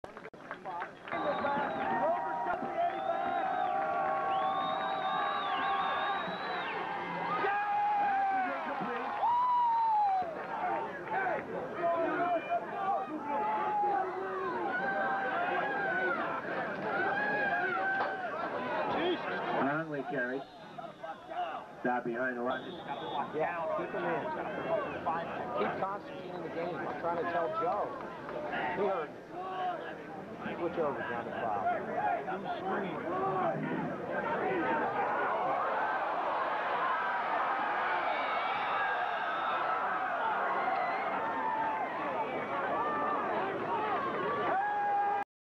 1993 WestConn vs Kean.
WCSU vs Kean 1993. Despite the pick6 WCSU still won.